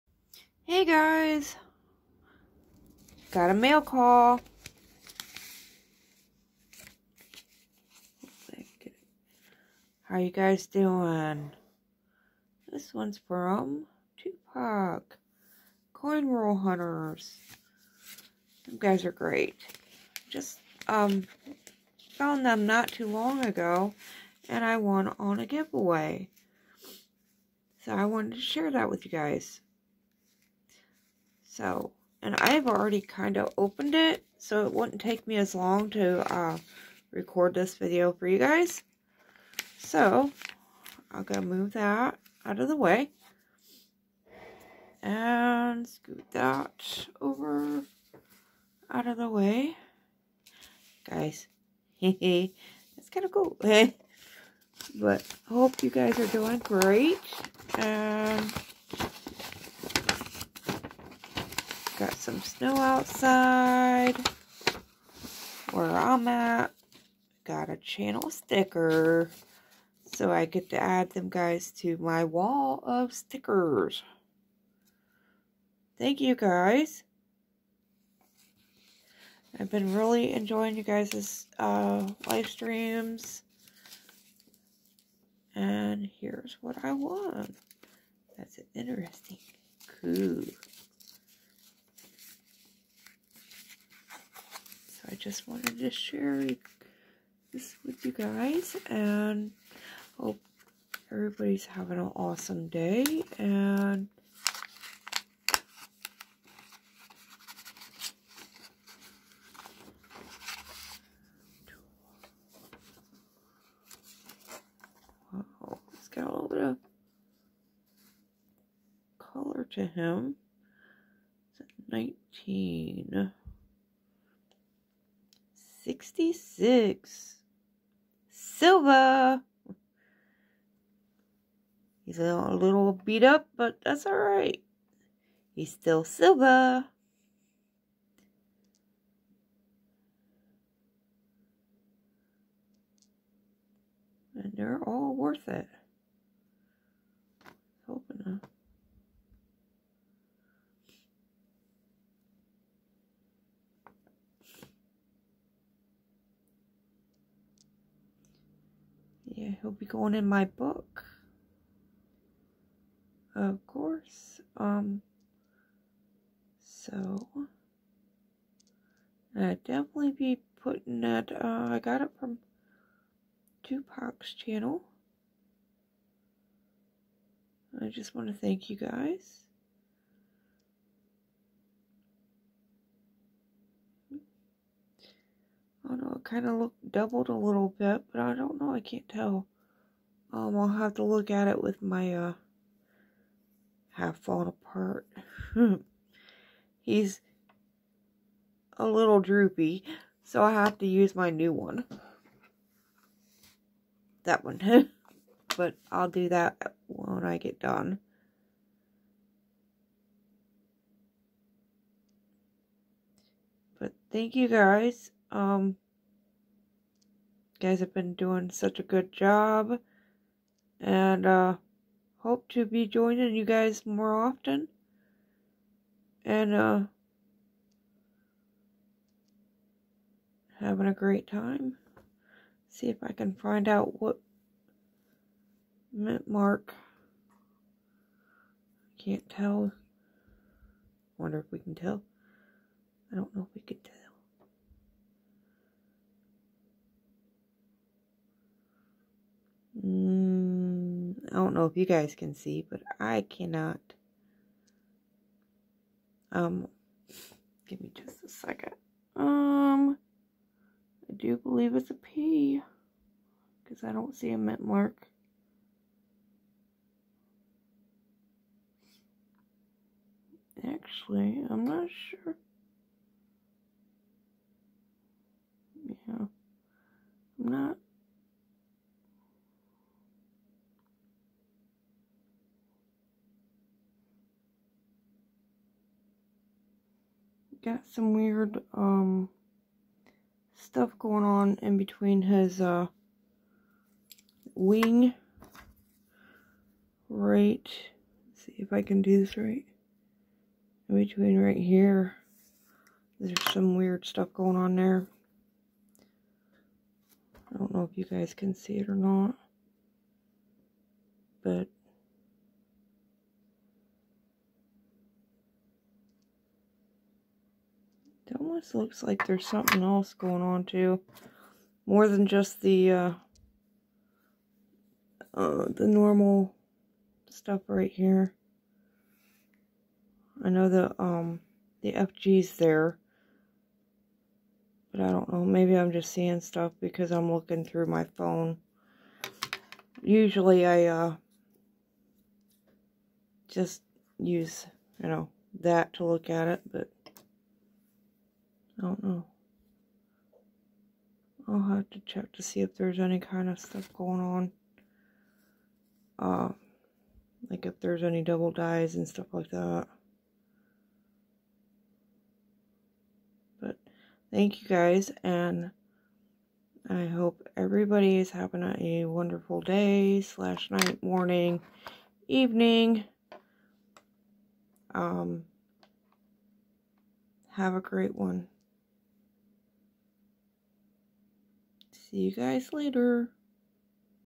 Shout Out & Mail 📬 Call!!
Opening 📬 mail from another channel. Thank you 2PAC CRH's channel for your channel and your just plain Awesomeness!! Winner winner chicken dinner(🐔🍲) of one of 2Pac CRH'S channel GAWs!!💜💜💜🌈🌈🐔🍲!! Sweet and it's not even Christmas 🎄🤣!!